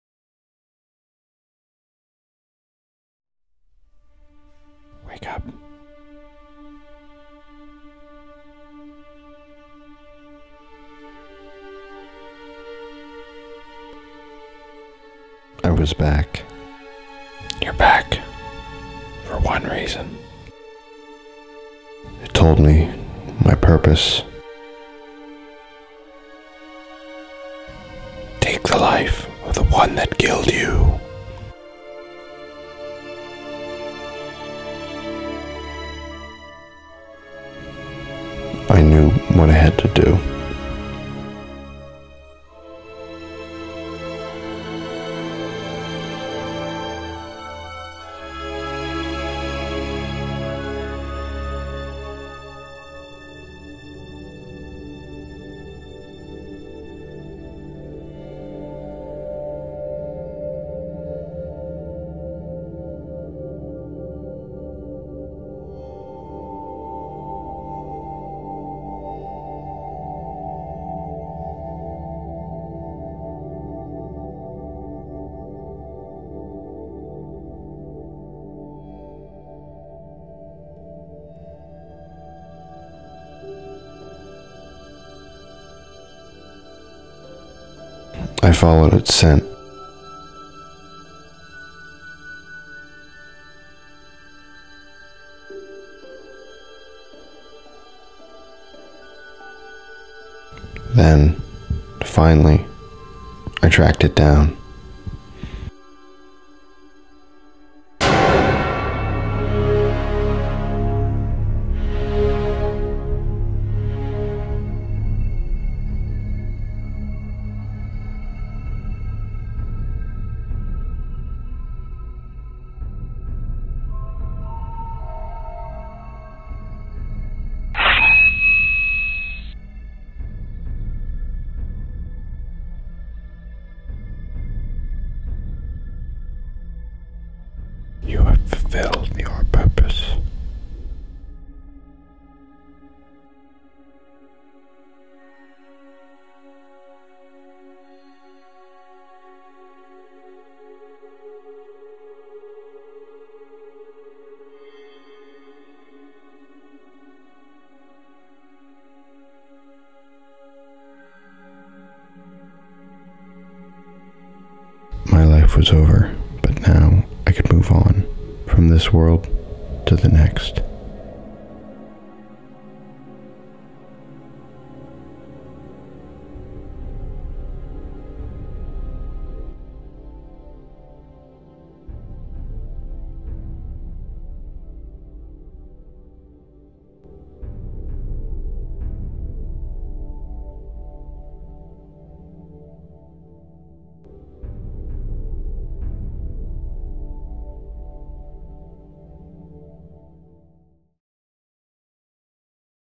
Hunter
This is the third film in my Hunter Trilogy.  The Hunter comes back to life and seeks revenge.  I created the film for an independent study course at Penn State Berks.  The outdoor scenes were shot at Blue Marsh and look beautiful.